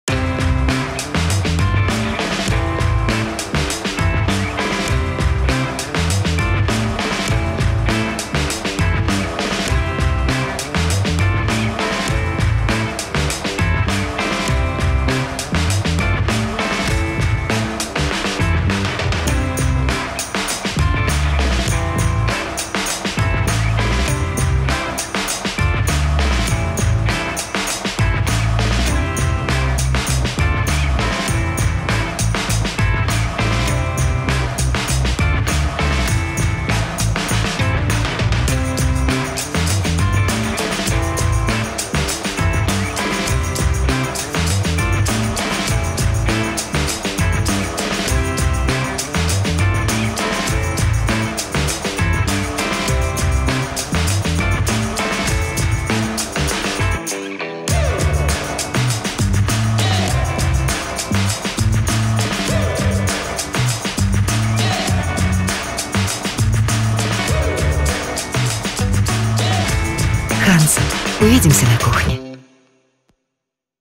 Новая функция SoftSteam в духовых шкафах Hansa
Высокий уровень влажности способствует поднятию и пропеканию теста изнутри. Так же эта функция рекомендуется для повторного нагревания блюд.
Подробнее о этих и других преимуществах новая функции SoftSteam  вы можете узнать, посмотрев это видео!
Увидимся на кухне!